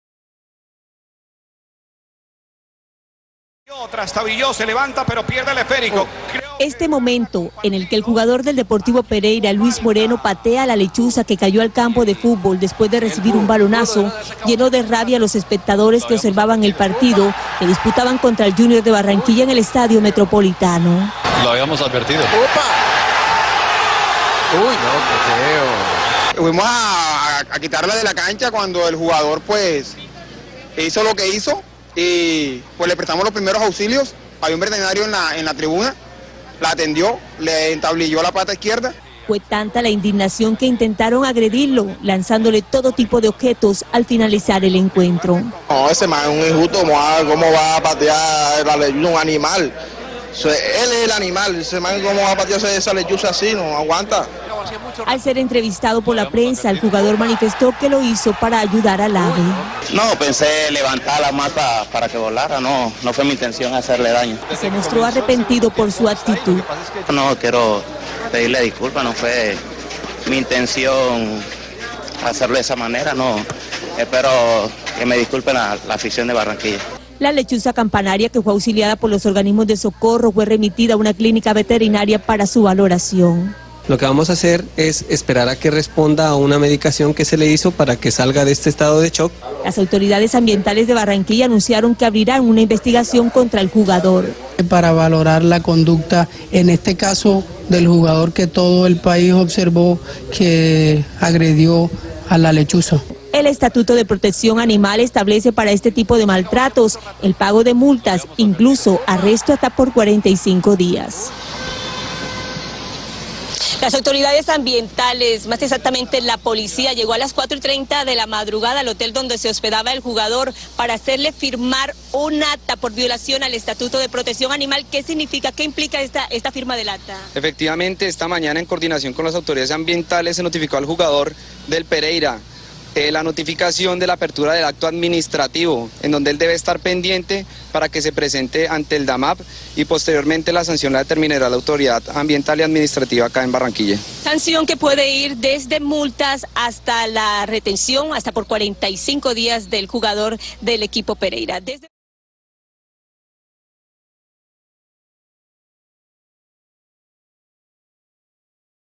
le scandal Luis Moreno - chouette
Luis Moreno s'est fait pas mal d'amis lors de son dernier match entre le Deportivo Pereira et Atletico Junior, le défenseur panaméen a shooté dans une chouette qui s'était crashée sur la pelouse.
Le footeux carrément débile risque tout de même trois mois de prison pour son geste, sachant que la chouette est morte malgré des soins prodigués dans une clinique vétérinaire